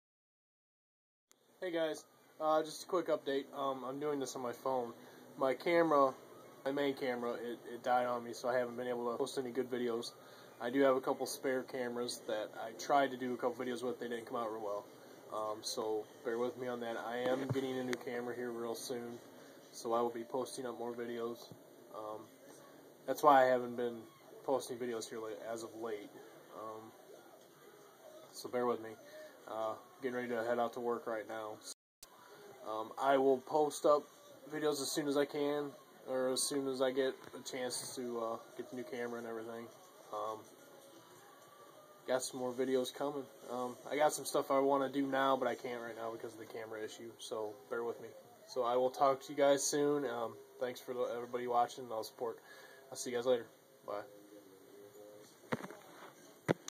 UPDATE
Hey guys just wanna let you know whats going on.  Been having camera issues and haven't been able to make any good videos. Getting a new one soon so i will be posting really soon. Thanks everyone for the support.